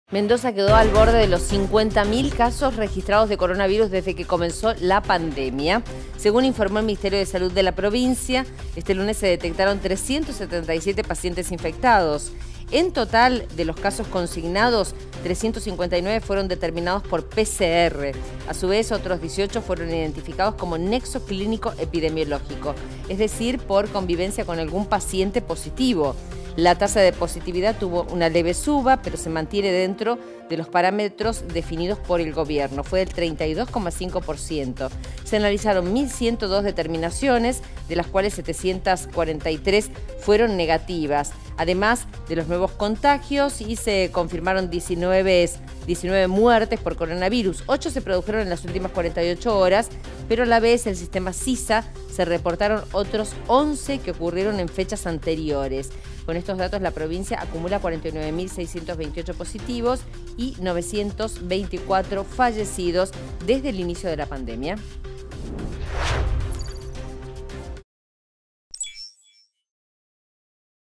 El Gobierno de Mendoza informó que se registraron 374 nuevos casos de coronavirus
Después de los anuncios del Gobernador, se realizaron 1.102 test en toda la provincia. Siguen sumando personas fallecidas por coronavirus de fechas anteriores.